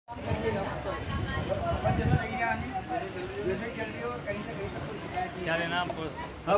Gaya Kotha 1
Pitru Paksha 2019: River Falgu revealed in Ujaain You get bless like Gaya